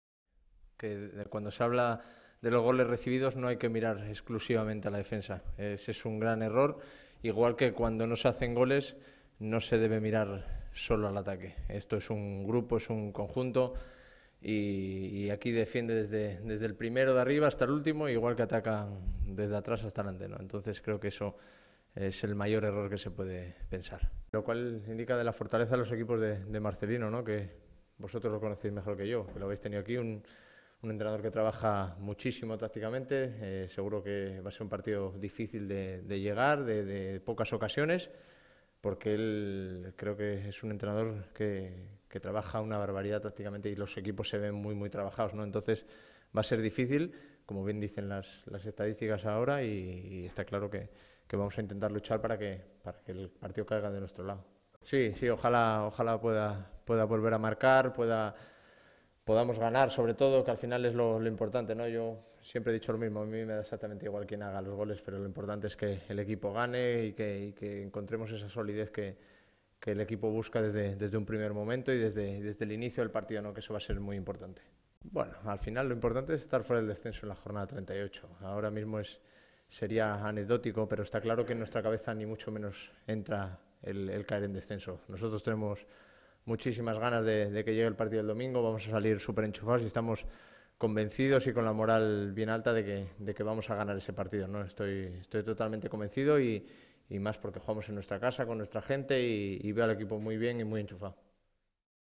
Luis García ve anecdótico poder caer en descenso
(24/11/2011) A tan sólo un punto del descenso, el Real Zaragoza debe vencer al Sevilla F.C. para evitar caer en la zona peligrosa. Sin embargo, Luis García cree que es anecdótico y se centra en ganar a los andaluces con el calor de La Romareda. El ovetense, además, resta responsabilidad a la defensa asegurando que no encajar es trabajo de todos.